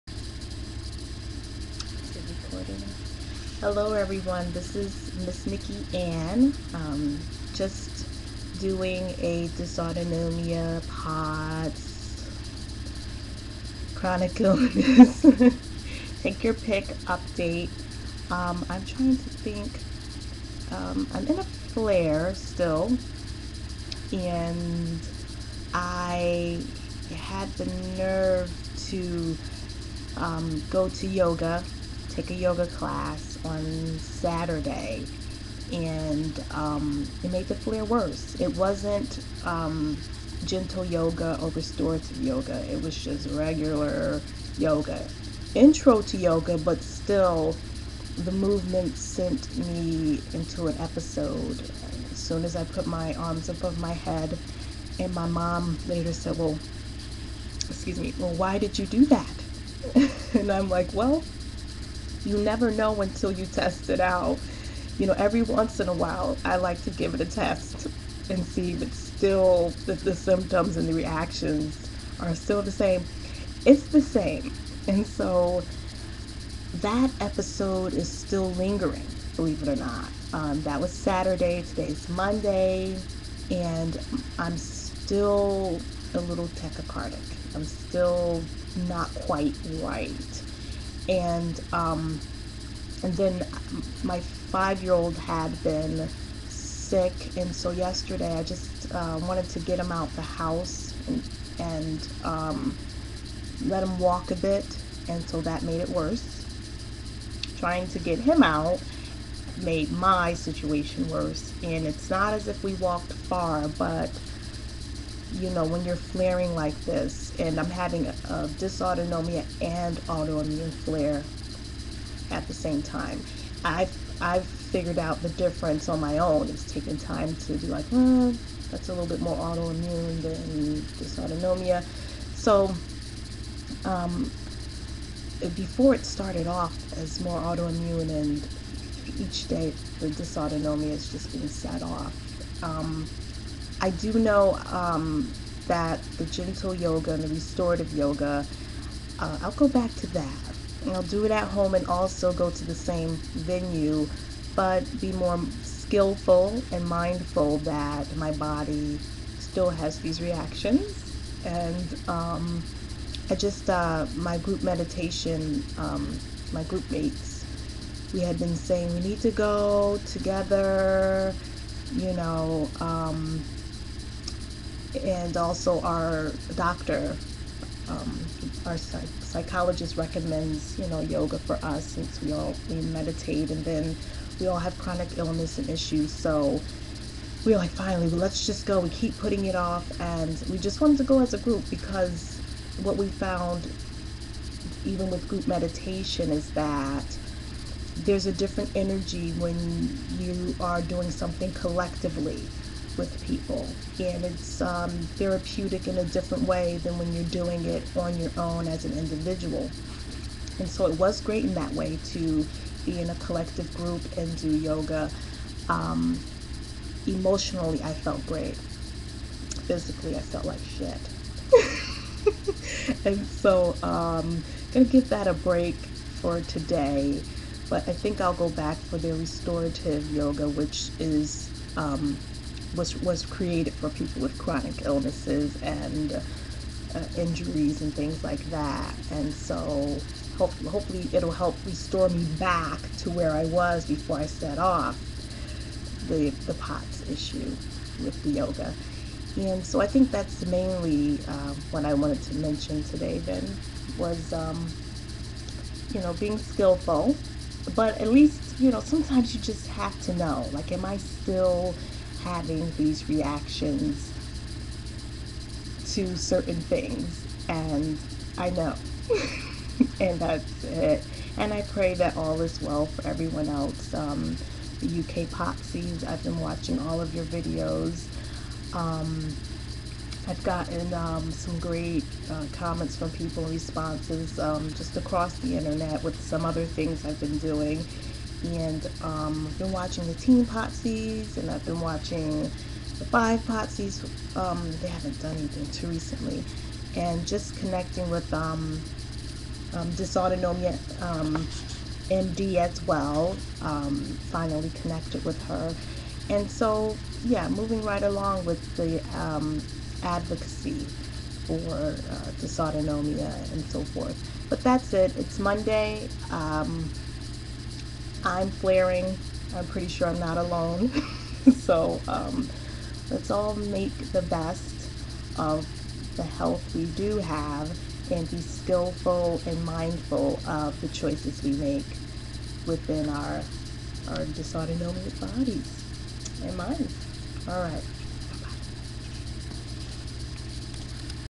Dysautonomia (POTS), Autoimmune Disorder and YOGA
MissNikkiAnn discusses how a recent Yoga class affected her POTS.